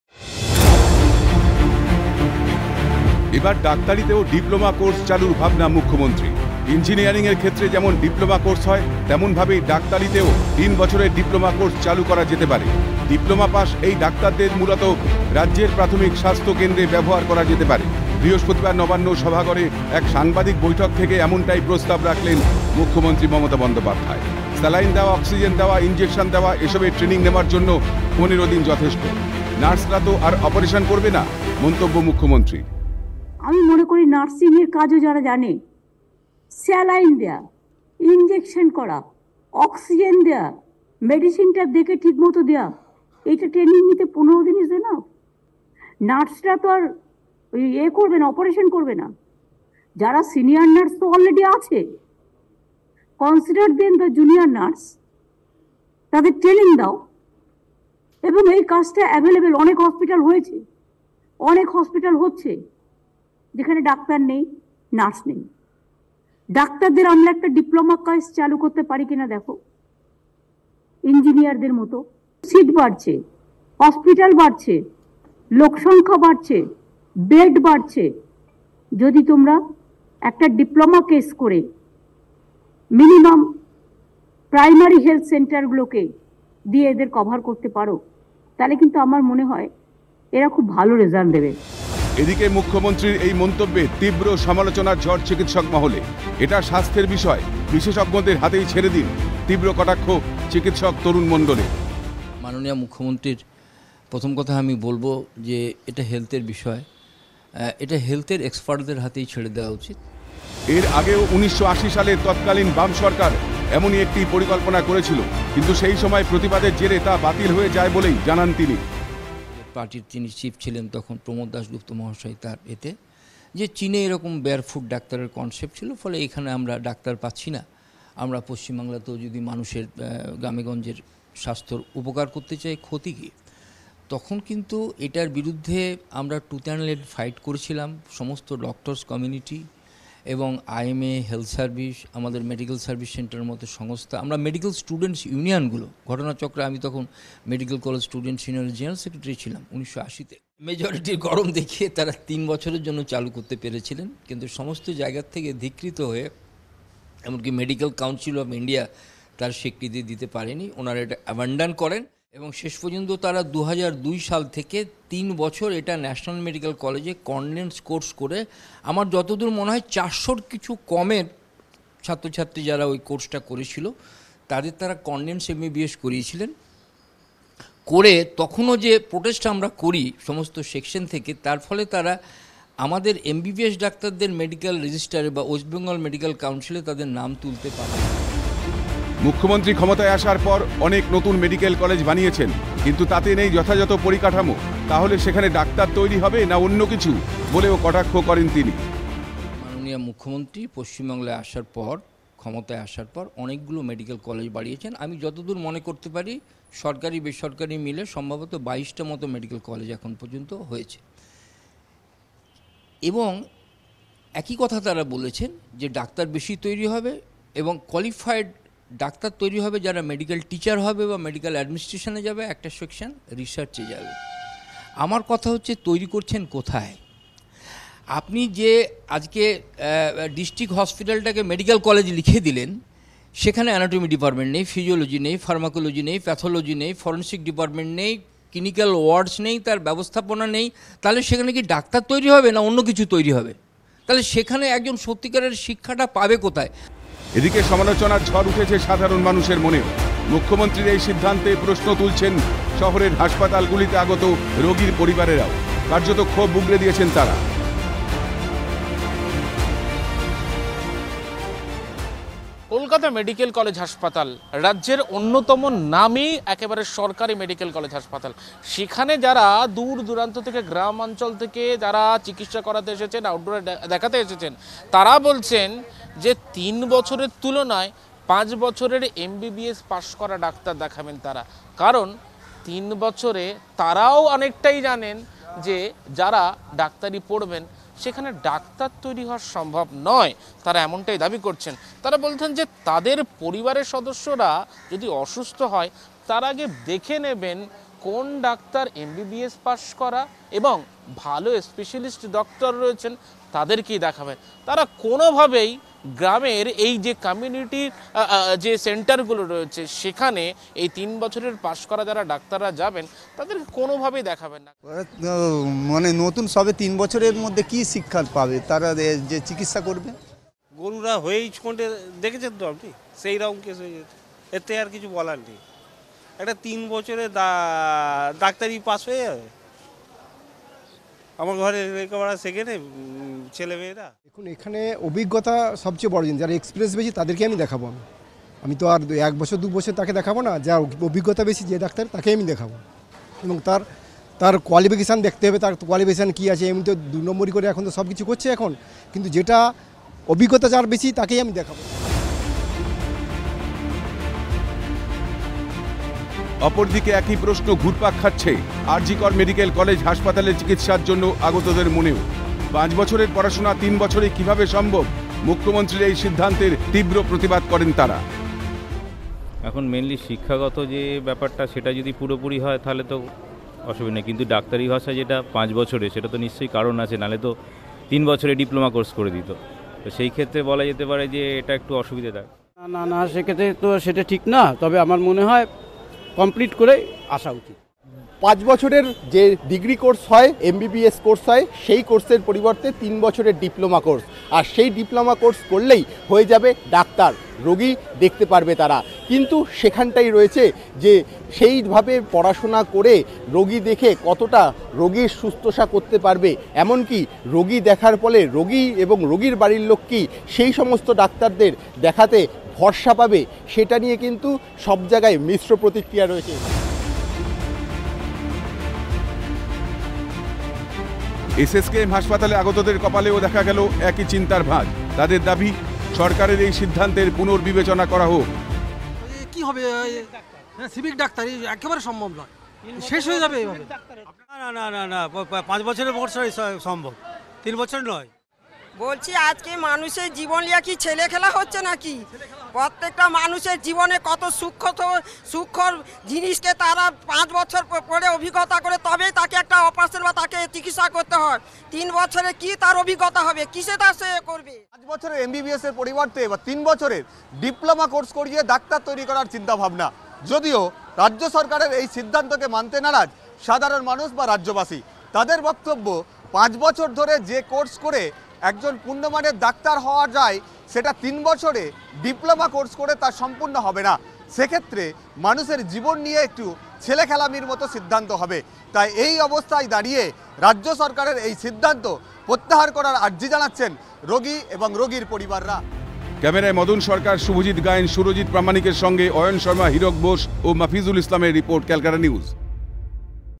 ডিপ্লোমা কোর্সে ডাক্তার! সমালোচনার মুখে মুখ্যমন্ত্রীর প্রস্তাব | CN
ইঞ্জিনিয়ারিংয়ের মতো ডাক্তারিতেও ডিপ্লোমা কোর্সের ভাবনা মুখ্যমন্ত্রীর। মুখ্যমন্ত্রীর এই ভাবনার পর থেকেই সমালোচনার ঝড় রাজ্যজুড়ে।